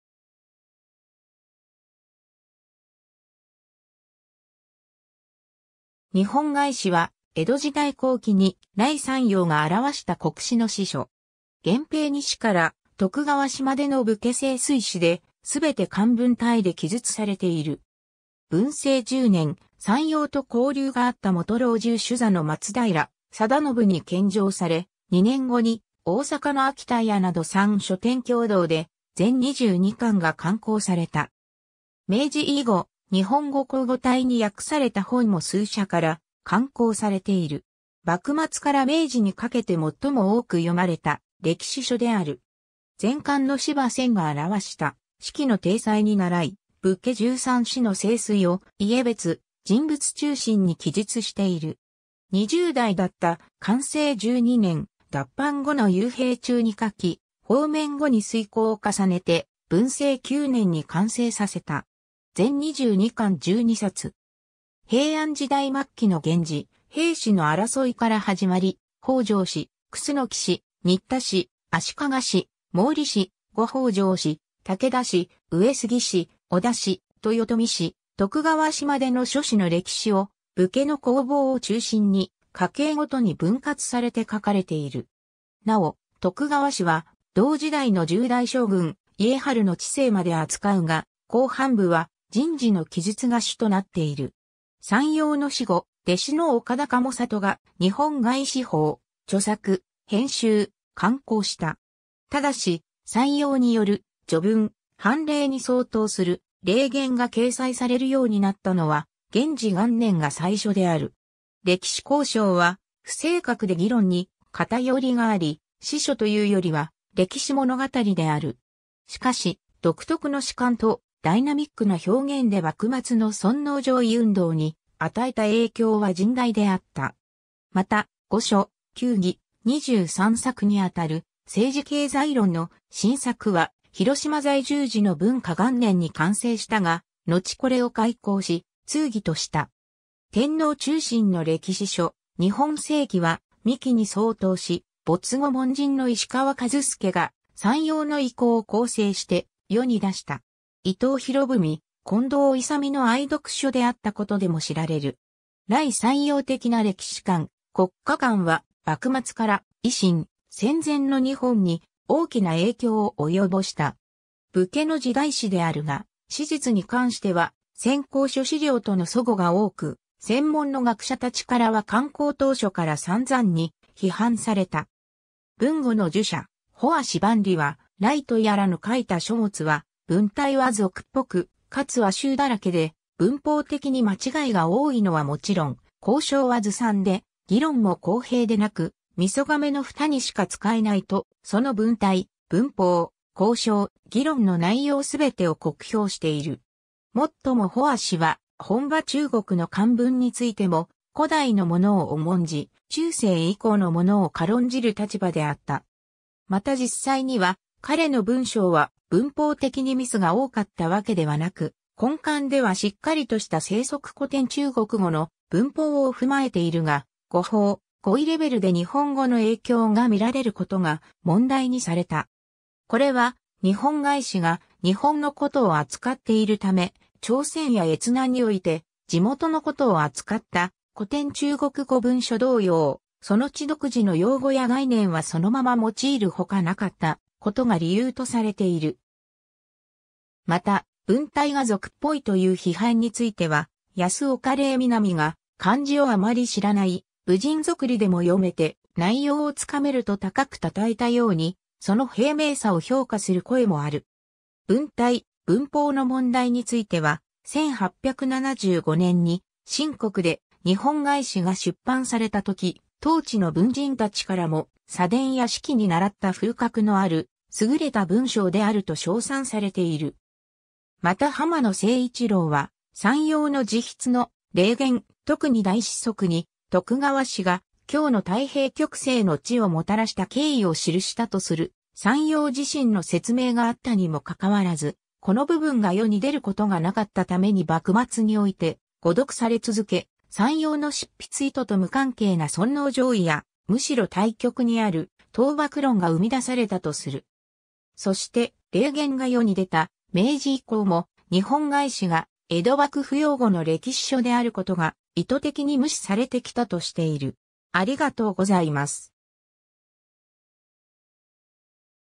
日本外史
『日本外史』（にほんがいし）は、江戸時代後期に頼山陽が著した国史の史書（外史とは民間による歴史書の意）。
源平2氏から徳川氏までの武家盛衰史で、すべて漢文体で記述されている。
文政10年（1827年）、山陽と交流があった元老中首座の松平定信に献上され、2年後に大坂の秋田屋など3書店共同で全22巻が刊行された。
明治以後、日本語口語体に訳された本も数社から刊行されている。
幕末から明治にかけてもっとも多く読まれた歴史書である。
 前漢の司馬遷が著した『史記』の体裁にならい、武家13氏の盛衰を家別・人物中心に記述している。
20代だった寛政12年（1800年）、脱藩後の幽閉中に書き、放免後に推敲を重ねて文政9年（1826年）に完成させた。
全22巻 12冊。
 平安時代末期の源氏・平氏の争いから始まり、北条氏・楠氏・新田氏・足利氏・毛利氏・後北条氏・武田氏・上杉氏・織田氏・豊臣氏・徳川氏までの諸氏の歴史を、武家の興亡を中心に家系ごとに分割されて（列伝体）書かれている。
なお「徳川氏」は、同時代の10代将軍家治の治世まで扱うが、後半部は人事の記述が主となっている。
山陽の死後、弟子の岡田鴨里が『日本外史補』を著作・編集・刊行した。
ただし、山陽による序文・凡例に相当する「例言」が掲載されるようになったのは、元治元年（1864年）が最初である。
 歴史考証は不正確で議論に偏りがあり、史書というよりは歴史物語である（後述）。
しかし、独特の史観とダイナミックな表現で幕末の尊皇攘夷運動に与えた影響は甚大であった。
また「五書・九議・二十三策」にあたる政治経済論の『新策』は、広島在住時の文化元年（1804年）に完成したが、のちこれを改稿し『通議』とした。
天皇中心の歴史書『日本政記』（全16巻）は「三紀」に相当し、没後門人の石川和介が、山陽の遺稿を校正して世に出した。
伊藤博文、近藤勇の愛読書であったことでも知られる。
頼山陽的な歴史観、国家観は幕末から維新、戦前の日本に大きな影響を及ぼした。
 武家の時代史であるが、史実に関しては先行諸史料との齟齬が多く、専門の学者たちからは刊行当初から散々に批判された。
豊後の儒者帆足万里は、「頼とやらの書いた書物は、文体は俗っぽく、かつ和臭だらけで文法的に間違いが多いのは勿論、考証は杜撰で、議論も公平でなく、味噌甕のふたにしか使えない」と、その文体、文法、考証、議論の内容すべてを酷評している。
もっとも帆足は本場中国の漢文についても、古代のものを重んじ、中世以降のものを軽んじる立場であった。
また実際には彼の文章は文法的にミスが多かったわけではなく、根幹ではしっかりとした正則古典中国語の文法を踏まえているが、語法、語彙レベルで日本語の影響が見られることが問題にされた。
これは『日本外史』が日本のことを扱っているため、朝鮮や越南において地元のことを扱った古典中国語文書同様、その地独自の用語や概念はそのまま用いるほかなかったことが理由とされている。
また、文体が俗っぽいという批判については、保岡嶺南が「漢字をあまり知らない武人俗吏でも読めて内容をつかめる」と高く称えたように、その平明さを評価する声もある。
文体、文法の問題については、1875年に清国で『日本外史』が出版されたとき、当地の文人たちからも「左伝や史記に倣った風格のある優れた文章」であると賞賛されている。
また濱野靖一郎は、山陽の自筆の「例言」、特に第四則に、徳川氏が今日の太平極盛の治をもたらした経緯を記した（すなわち江戸幕府の正統性を主張した）とする山陽自身の説明があったにも関わらず、この部分が世に出ることがなかったために幕末において「誤読」され続け、山陽の執筆意図と無関係な尊王攘夷や、むしろ対極にある討幕論が生み出されたとする。
そして、「例言」が世に出た明治以降も『日本外史』が江戸幕府擁護の（裏を返せば討幕に否定的な）歴史書であることが意図的に無視されてきたとしている。